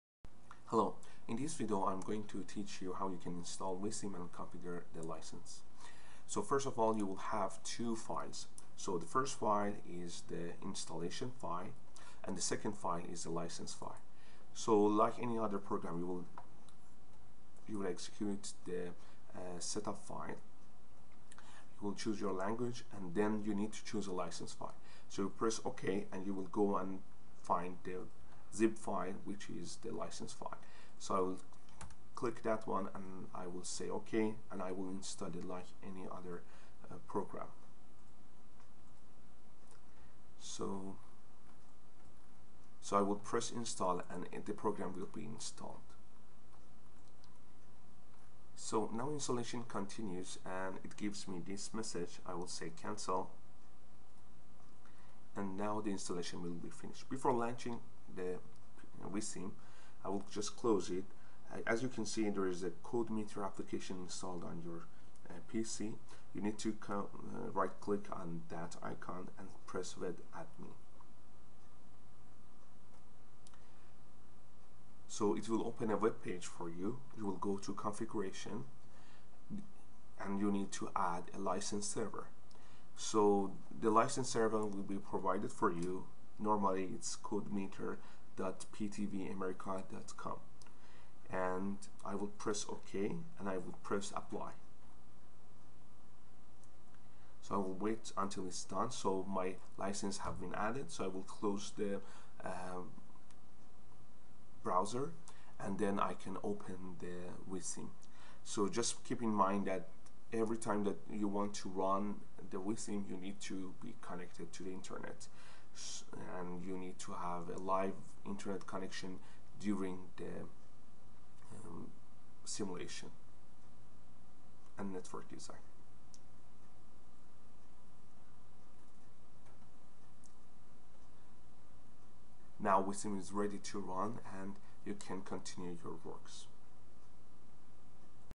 VISSIM Installation
This a tutorial on how you can install the VISSIM on your PC